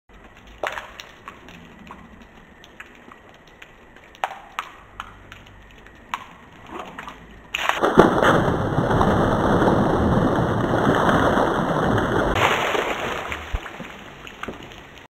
Drop a Water Balloon - muDDle
Drop a Water Balloon short video, Canon 550D shot, after effect edited. Made in India